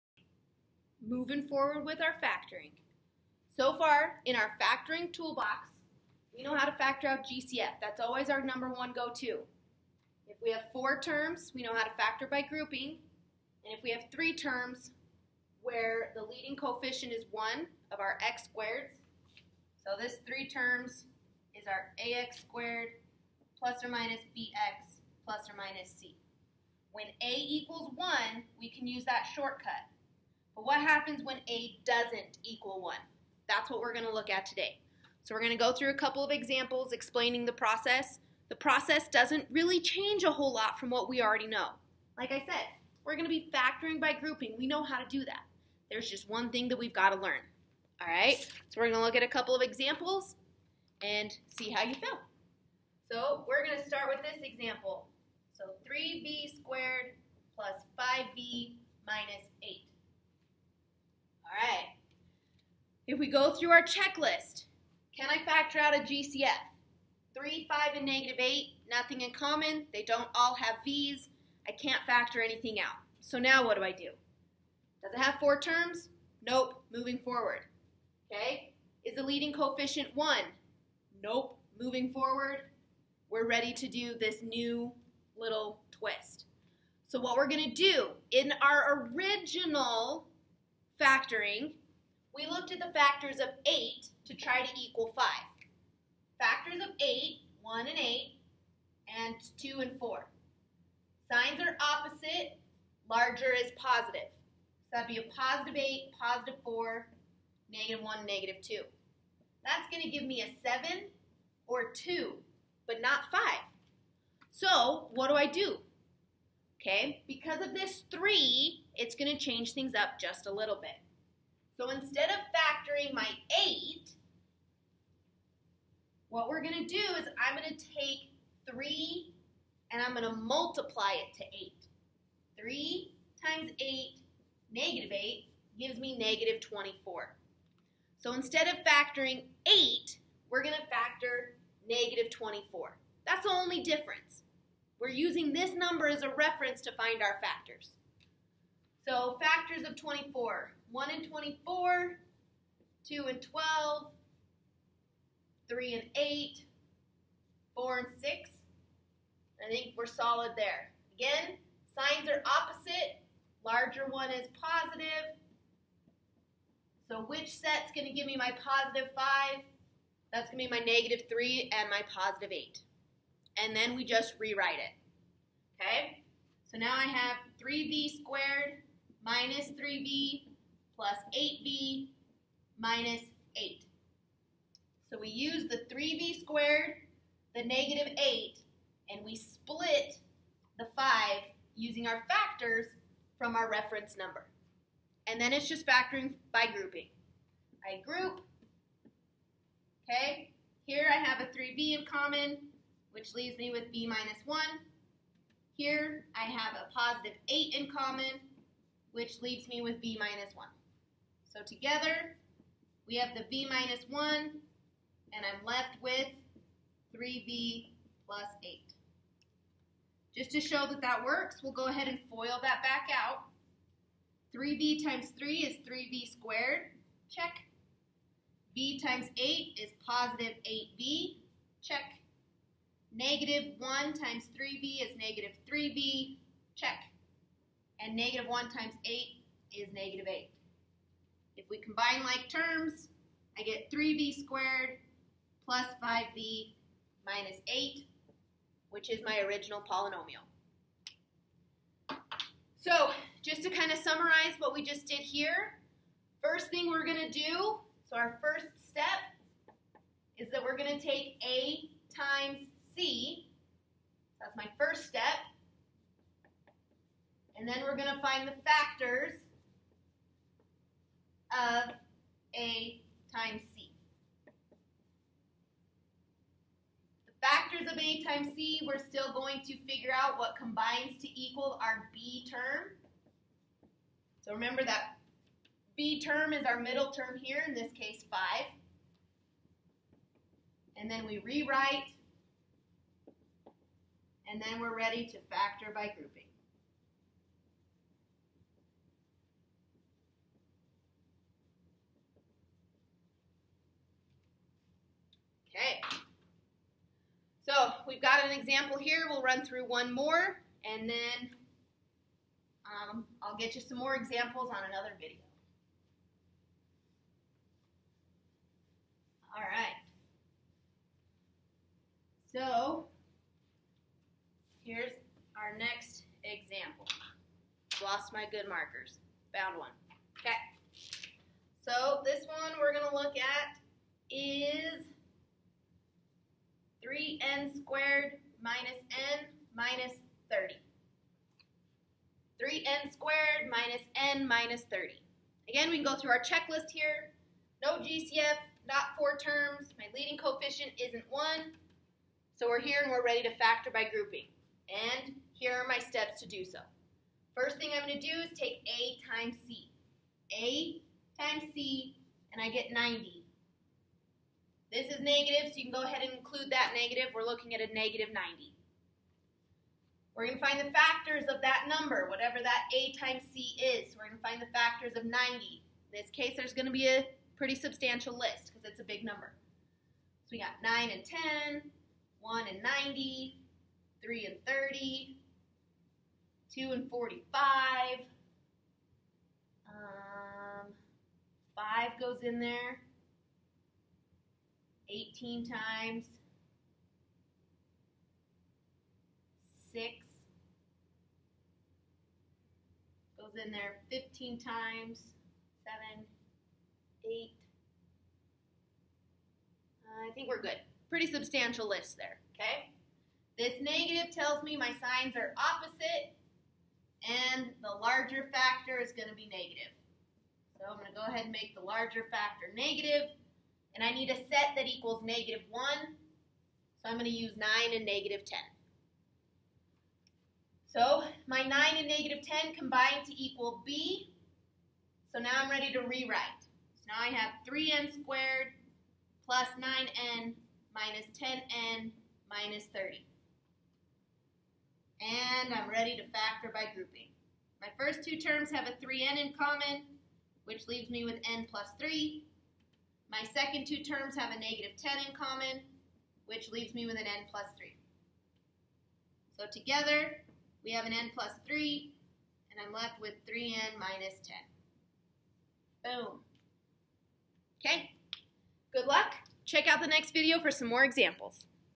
Factoring A x Squared
How to factoring a quadratic when the leading coefficient is not ONE.